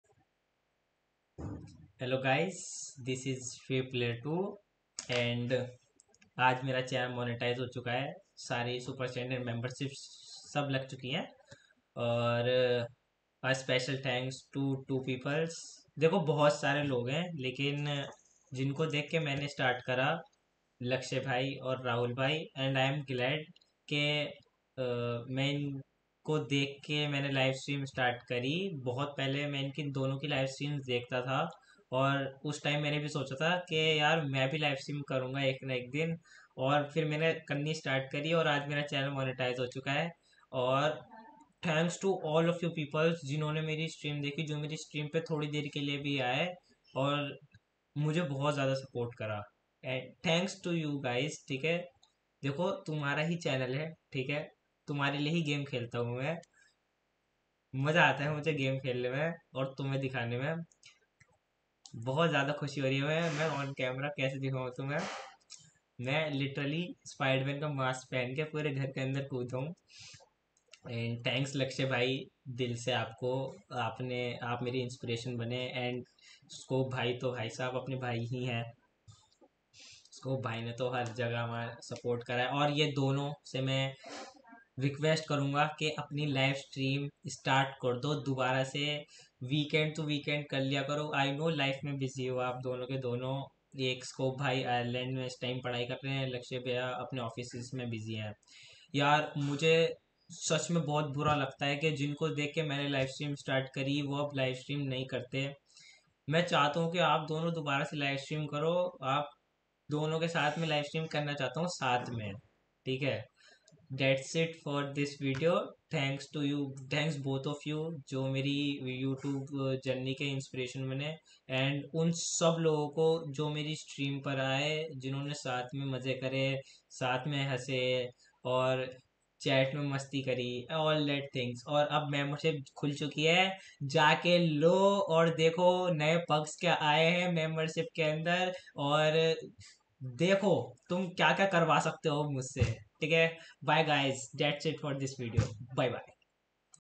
Special Thanks to
hello guys , player 2 this side
I started my yt journey becuase of these two guys @LakshayGameplays and @ScopeOP , both of them are my inspiration as well as motivates me a lot , I am glad that I am the first one who got monetized from there chats